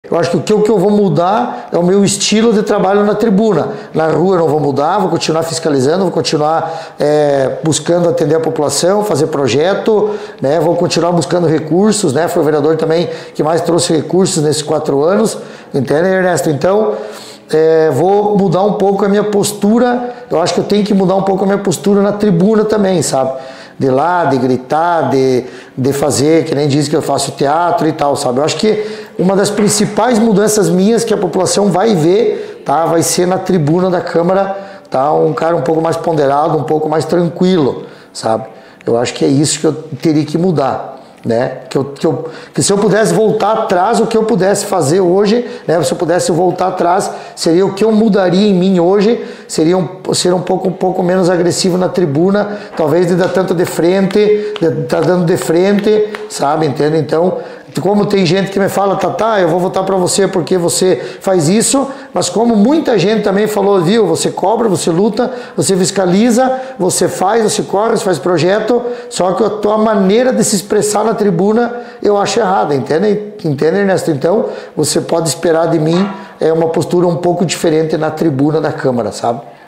Jonathan dos Santos é reeleito vereador em Videira
Outro vereador reconduzido ao cargo nesta eleição em Videira é Jonathan dos Santos, o Tata. Ele está filiado ao Uniao Brasil e recebeu 703. Conhecido por ser um parlamentar truculento ao usar da tribuna livre, garantiu à reportagem que pretende atuar de forma diferente a partir de agora, sendo mais polido em seus posicionamentos.